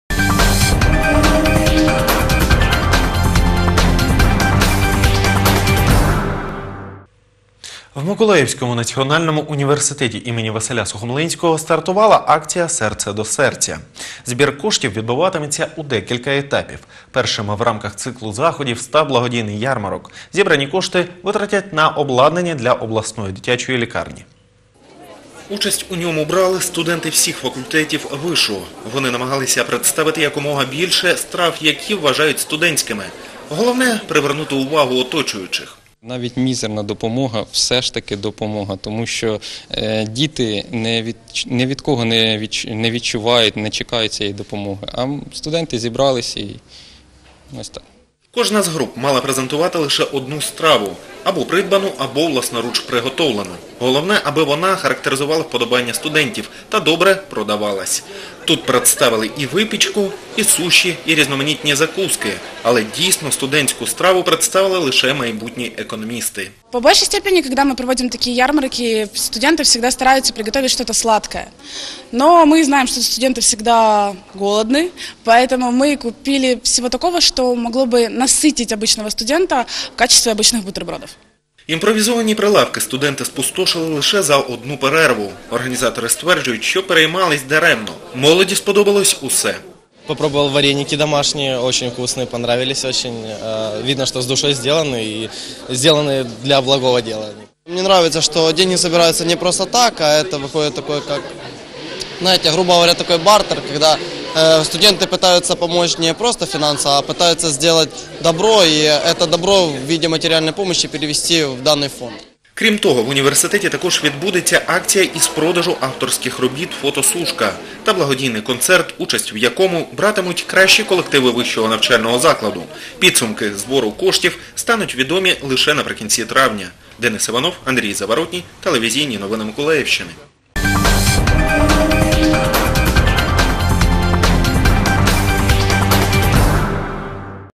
Благодійний ярмарок у МНУ ім.В.Сухомлинського
У МНУ ім. В.Сухомлинського відбувся благодійний ярмарок. Зібрані кошти спрямують на закупівлю медичного обладнання для обласної дитячої лікарні.
Автор: Денис Іванов.